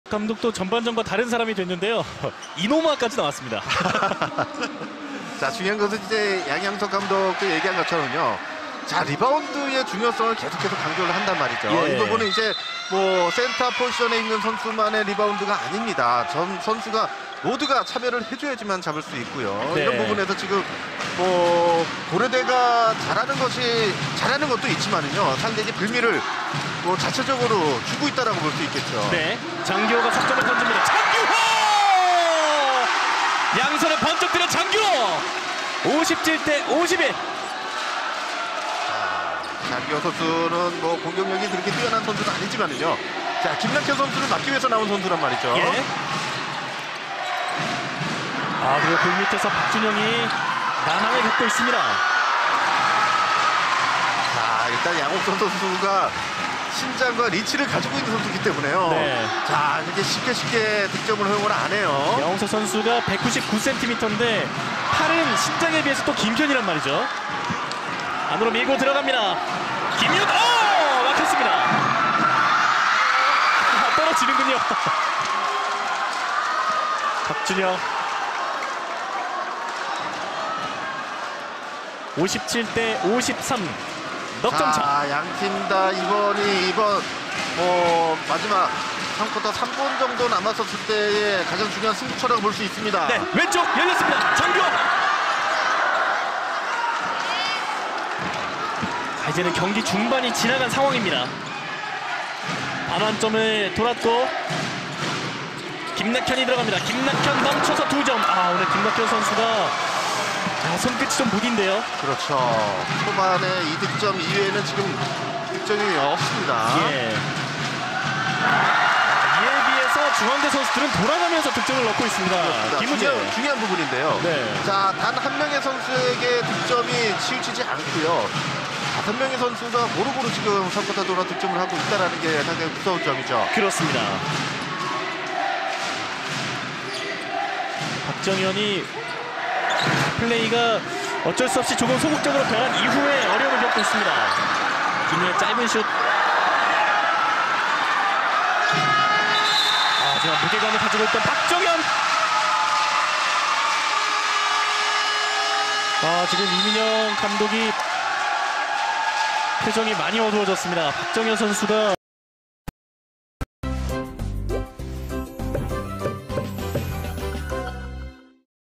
대학농구 - 돌아가며 골고루 점수 내는 중앙대 선수들. 20170622
돌아가며 골고루 점수 내는 중앙대 선수들